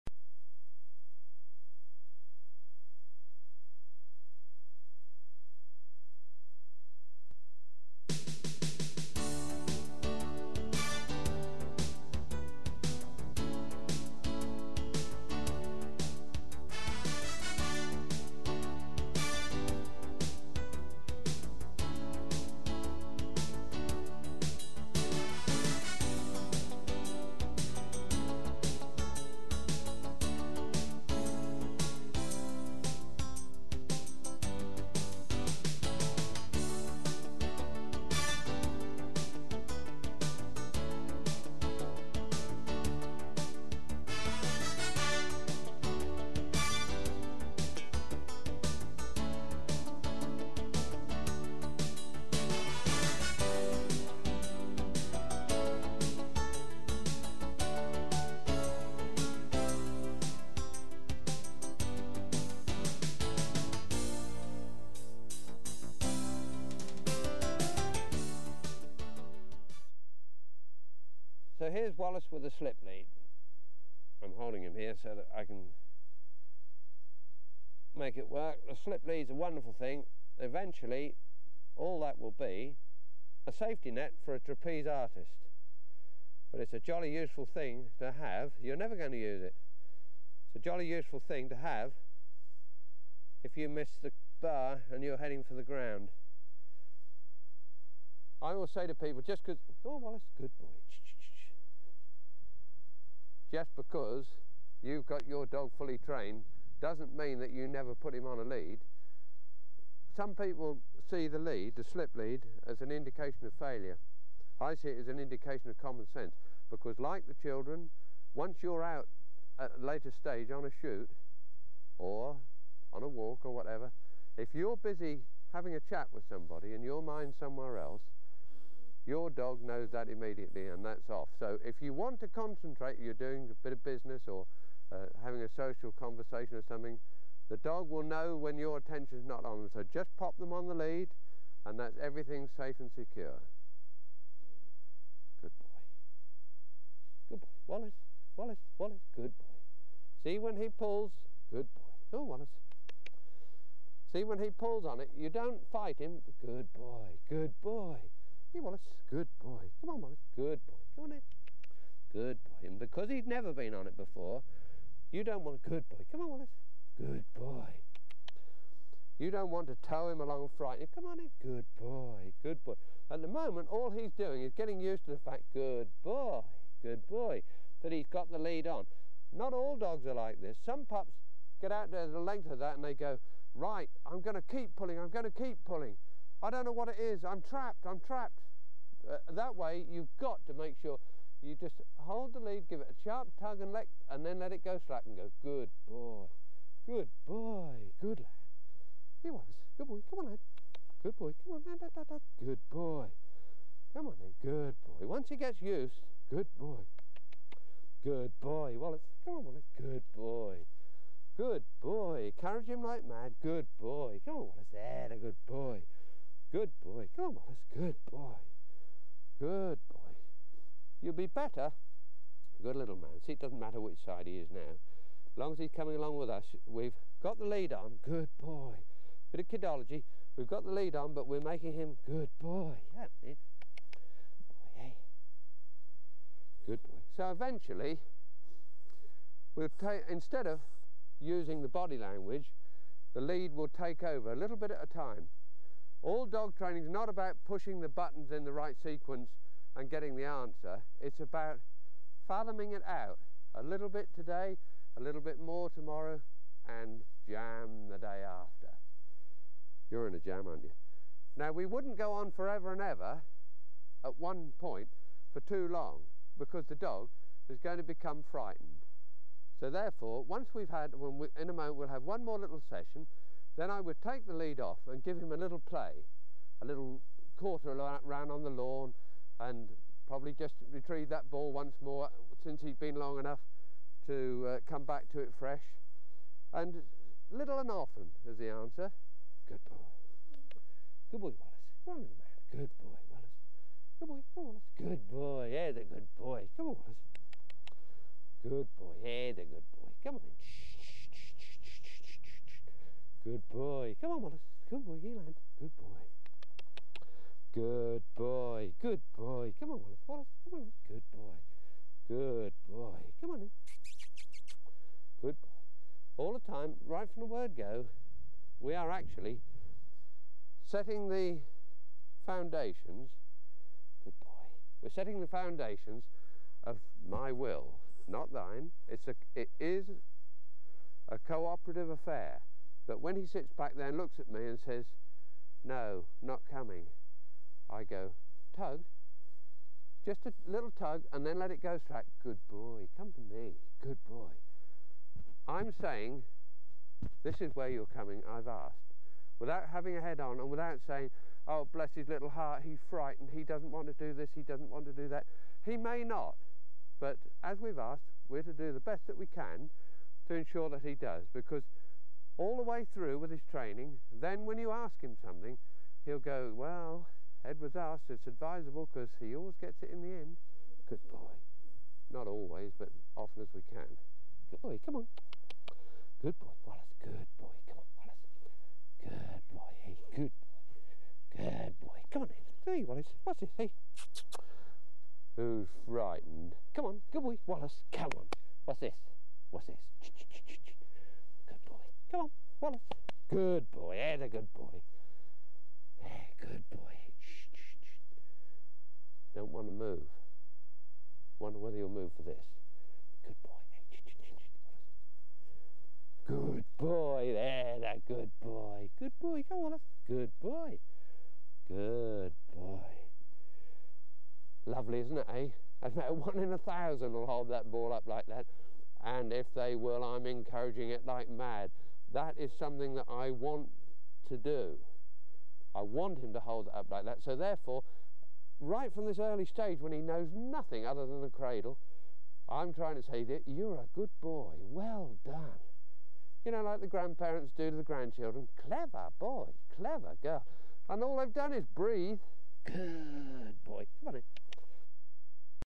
Sporting Scene - Cocker Spaniel Training Master Class P1 .
The Cocker Spaniel: Springers are Spaniels, but Cockers are all little
people, so many different little characters. The Cocker Spaniel as a breed is so unpredictable, a genie, a willo the wisp, difficult to pin down to reality, but such a rewarding little person,
just as you are feeling exasperated with some aspect of their work, they will do some remarkable thing that will leave you smiling and shaking your head in amazement! The Cocker, more than any other Spaniel, has the ability and character to lift your spirits if you are not feeling on cloud nine. Pound for pound they are more tenacious than others, Cockers have great stamina and seem able to work well even in warmer weather. Lovely companions and great little workers.
Contents:- The Puppy, play training, Introduction to lead, Equipment & whistle commands, Walking on lead, Introduction to transport, Walking off lead, Family dog, Transport, Basic training, Quartering, Dummy retrieve, Introduction to jumping fence, Retrieving over fence, Retrieving.